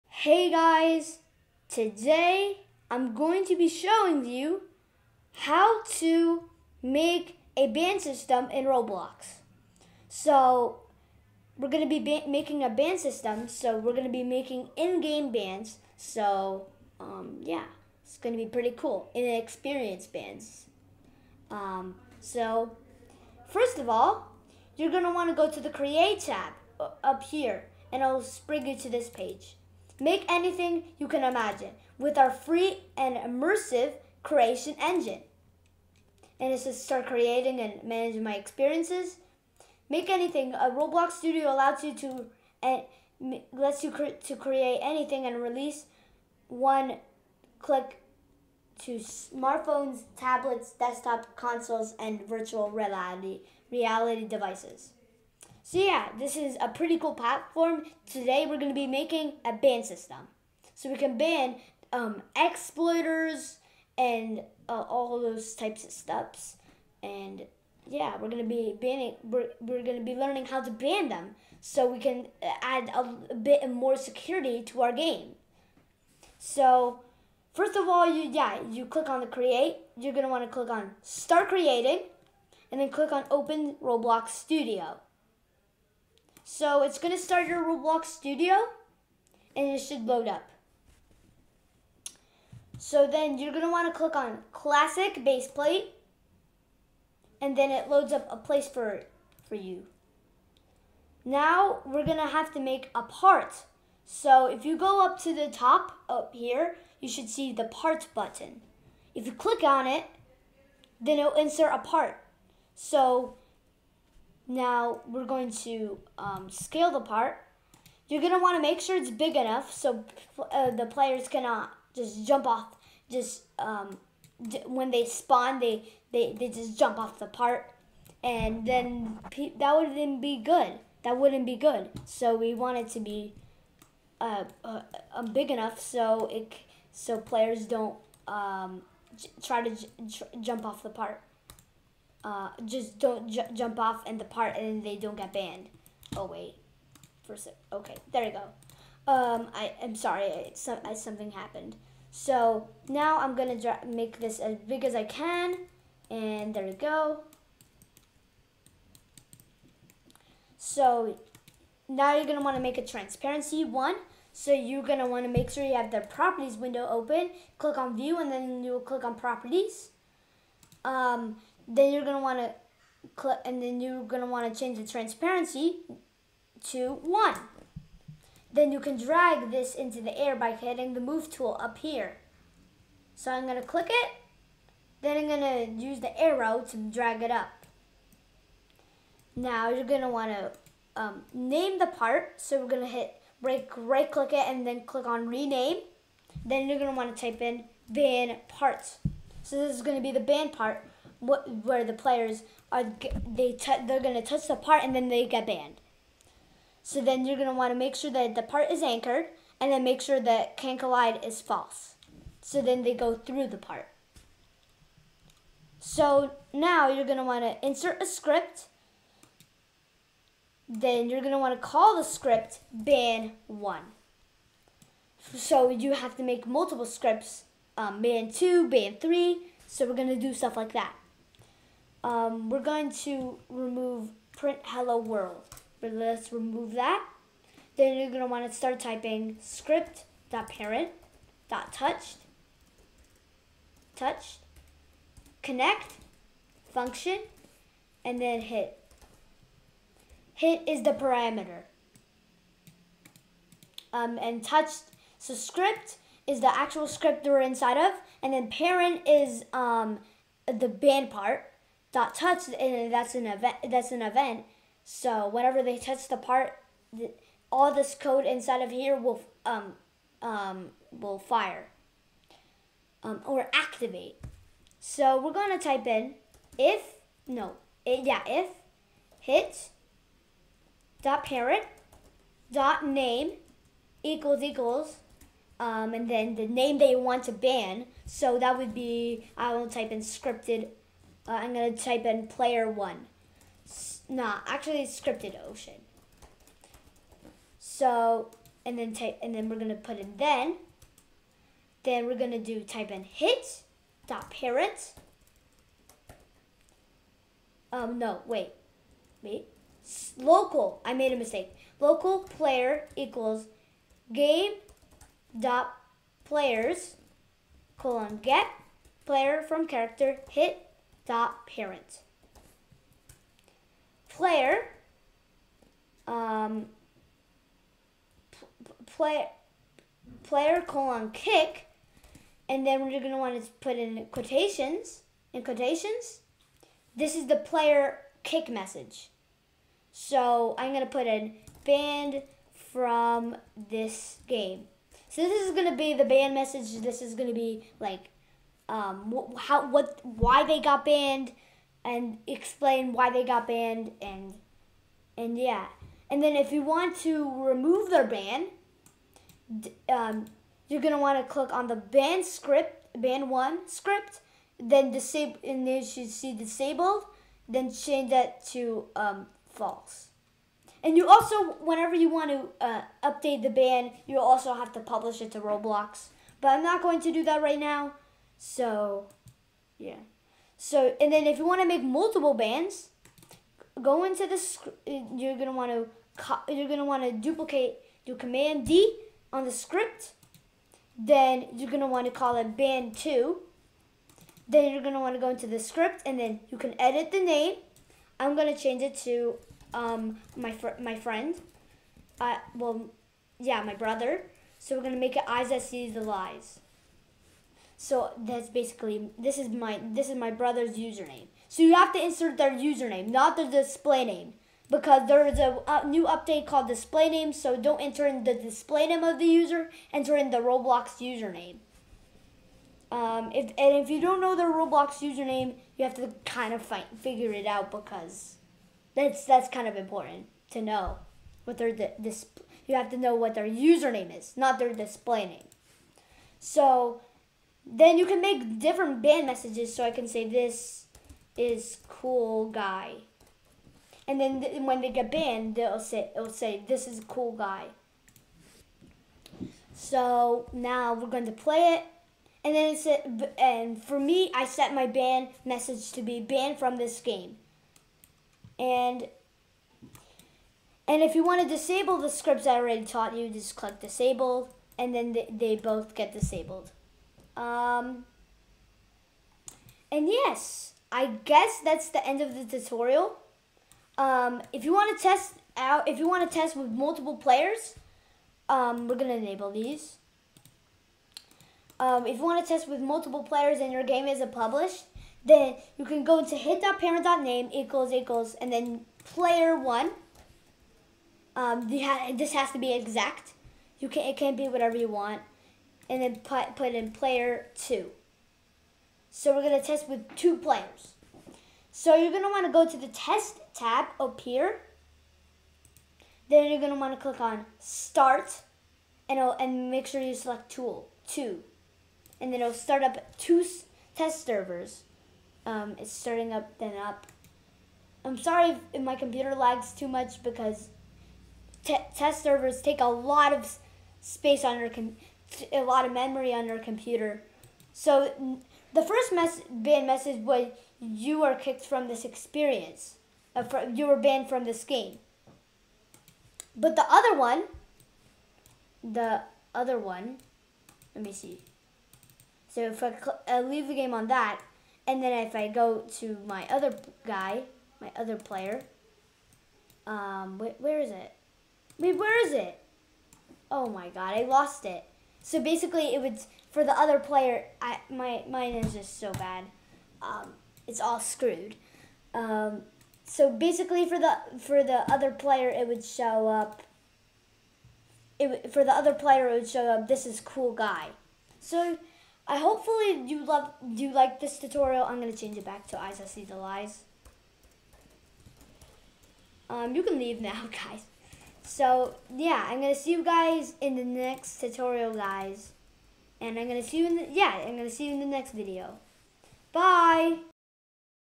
How to make a ban system in Roblox Studio
Hello guys today I will show you how to make a ban system on roblox!

Roblox Username: ScriptedOcean